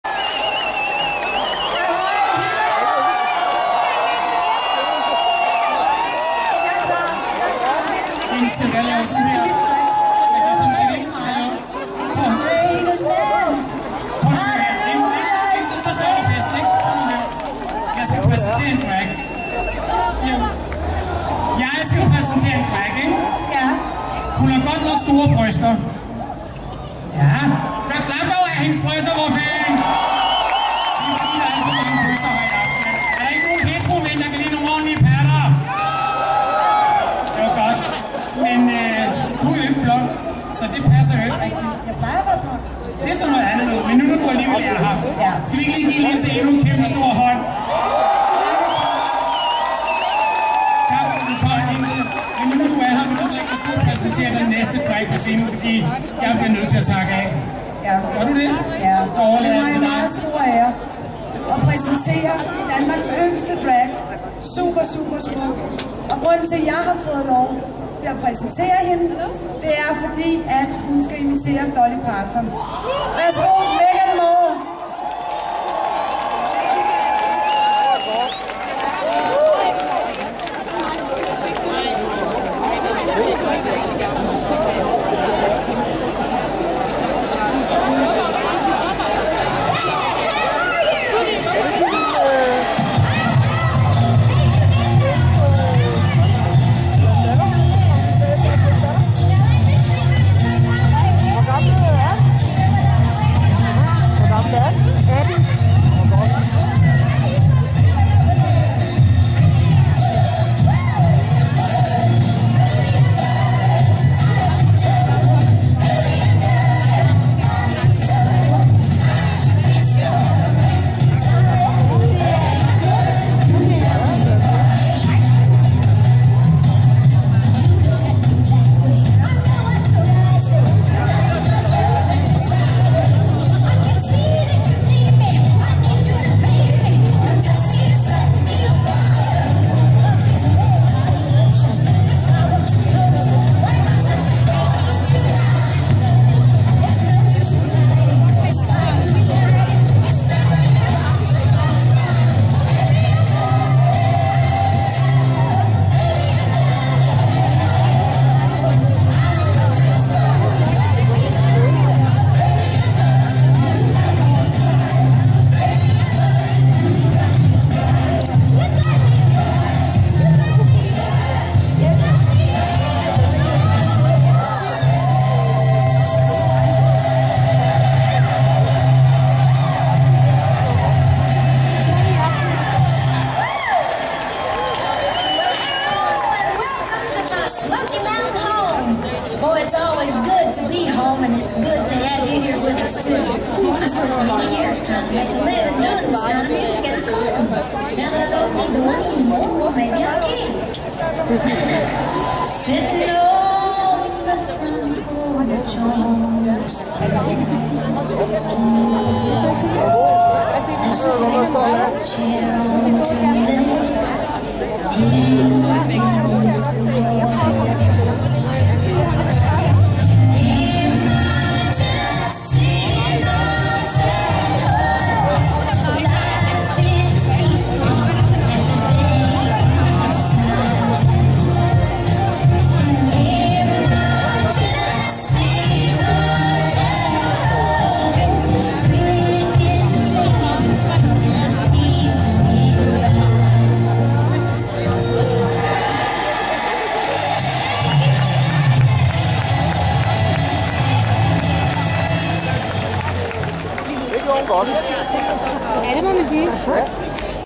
Drag Night 2013 - Megan Moore og Linse Kessler
Linse Kessler præsenterede min imitation af Dolly Parton - Tusind tak!